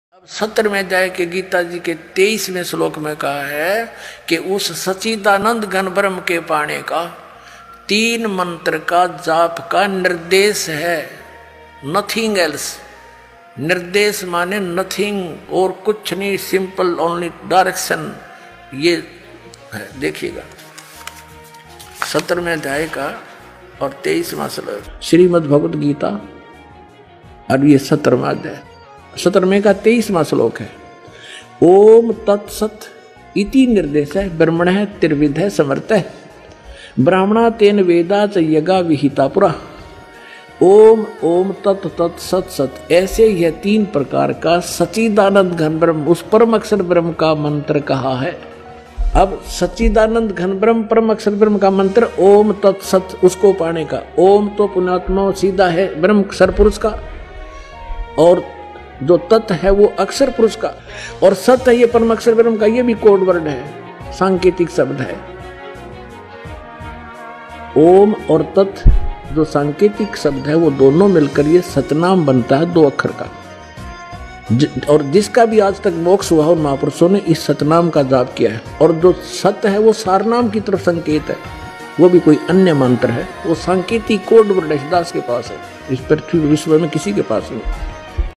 ओम, तत, सत सांकेतिक पूर्ण परमात्मा के नाम स्मरण का आदेश है। Sant Rampal Ji Maharaj
Mystery GYAN by Sant Rampal ji maharaj in Geeta book.